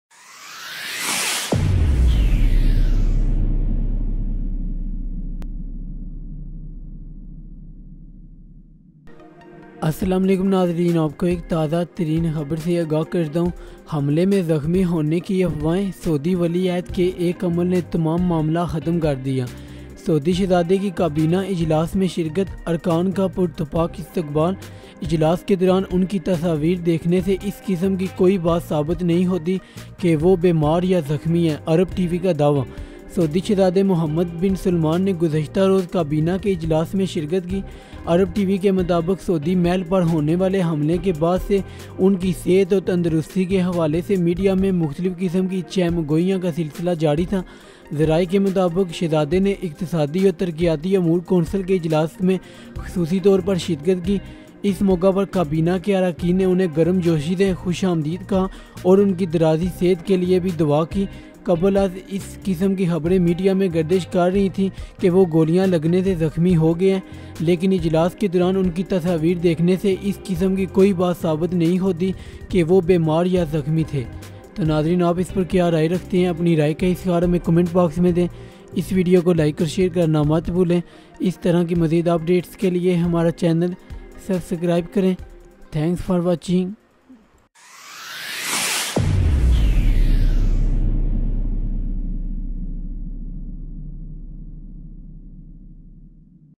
Latest News About Saudi Prince Mohammed Bin Salman
Mohammad bin Salman bin Abdulaziz Al Saud , known colloquially as MBS is the Crown Prince of Saudi Arabia, also serving as First Deputy Prime Minister, President of the Council for Economic and Development Affairs and Minister of Defense—the world's youngest office holder at the time. He has been described as the power behind the throne of his father, King Salman. He was appointed Crown Prince in June 2017 following his father's decision to remove Muhammad bin Nayef from all positions, making Mohammad bin Salman heir apparent to the throne.He has led several successful reforms, which include regulations restricting the powers of the religious police, and the removal of the ban on female drivers. Further cultural developments under his reign include the first Saudi public concerts by a female singer, the first Saudi sports stadium to admit women, and an increased presence of women in the workforce.